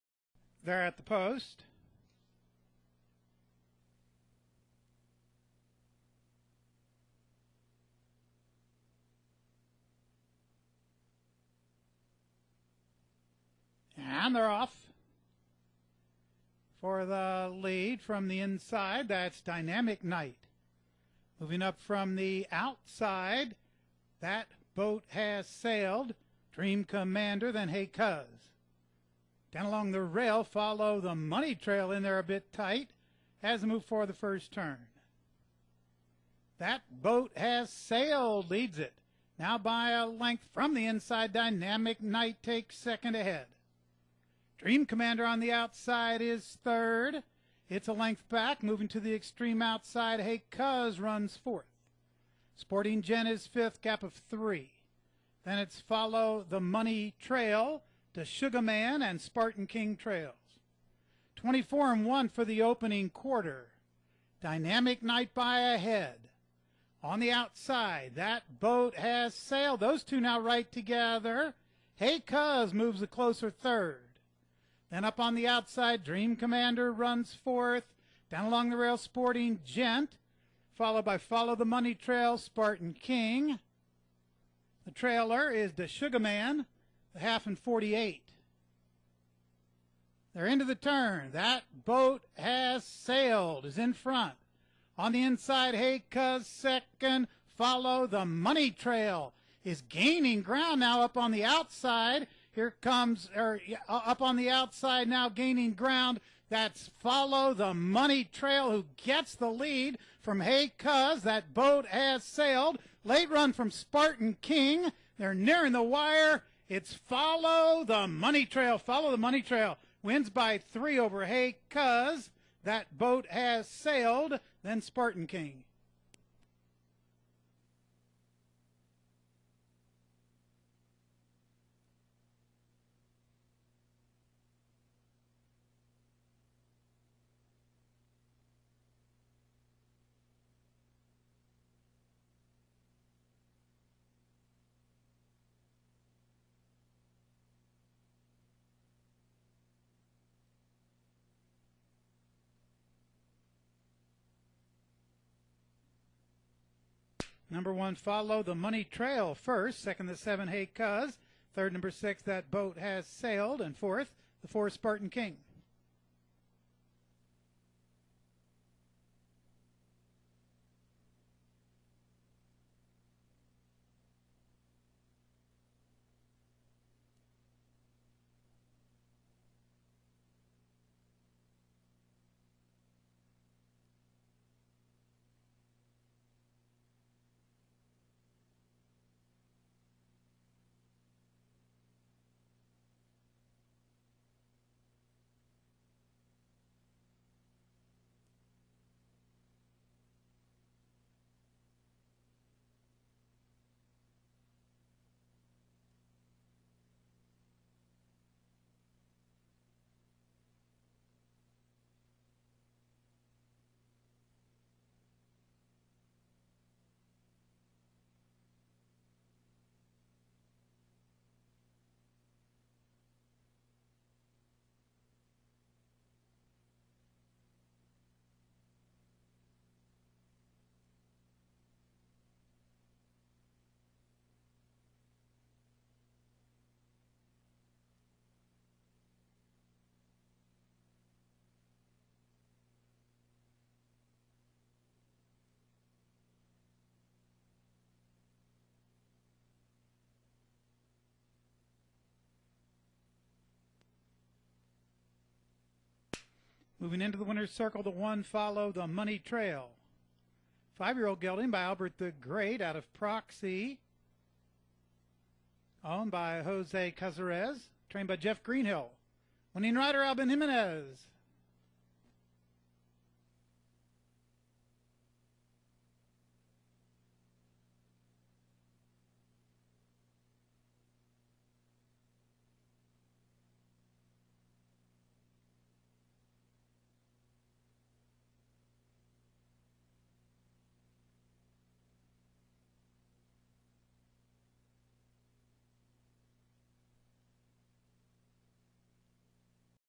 Folowthemoneytrail Wins at Turfway Park for Trainer Jeff Greenhill
FOLOWTHEMONEYTRAIL reserved early, made a run three wide on the second turn, challenged once in the stretch and drew off under steady urging. HEY CUZ forwardly placed four wide, challenged on the second turn, was no match for the winner but held on for the place. THATBOATHASSAILED sprinted clear, alternated for the lead after a quarter, stayed out in the three path on the second turn, held on well to the final furlong and tired. SPARTAN KING outrun for a half, raced in the three path on the second turn and showed late interest. DA SUGA MAN well back to the stretch, improved his position while no threat. DYNAMIC KNIGHT stumbled at the start, made a quick recovery, joined the leader after a quarter, vied for the lead for nearly six furlongs and faded. DREAM COMMANDER forwardly placed three wide, moved out a path on the second turn and faded in the stretch. SPORTING GENT drifted out a bit once in the first turn, raced close up to the three furlong marker and stopped.